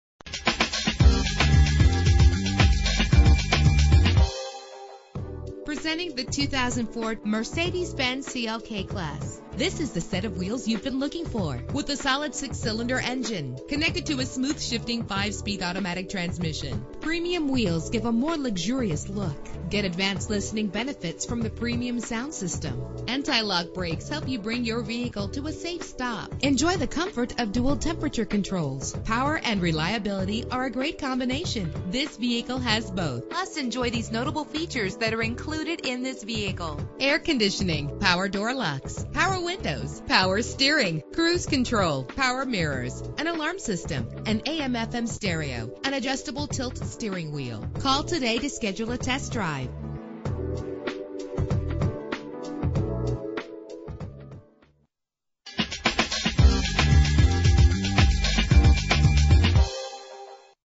2004 Mercedes-Benz CLK-Class - Orlando FL
Year: 2004
Make: Mercedes-Benz
Model: CLK-Class
Trim: CLK320
Engine: 3.2 liter 6 cylinder 18 valve
Transmission: 5-Speed Automatic
Color: Desert Silver Metallic
Mileage: 53332
Address:
7040 Colonial Dr